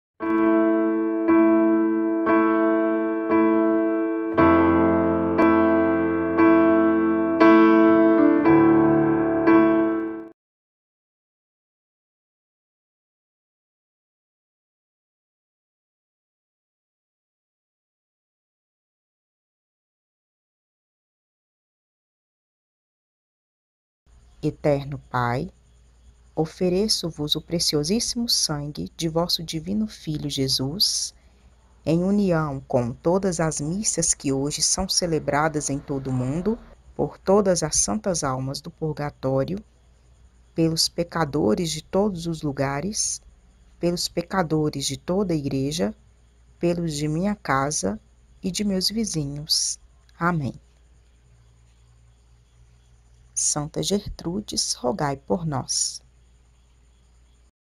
Oração de Santa Gertrudes pelas almas do purgatório
Eterno Pai, Ofereço-Vos o Preciosíssimo Sangue de Vosso Divino Filho Jesus, em união com todas as Missas que hoje são celebradas em todo o mundo; por todas as Santas almas do purgatório, pelos pecadores de todos os lugares, pelos pecadores de toda a Igreja, pelos de minha casa e de meus vizinhos. Amém!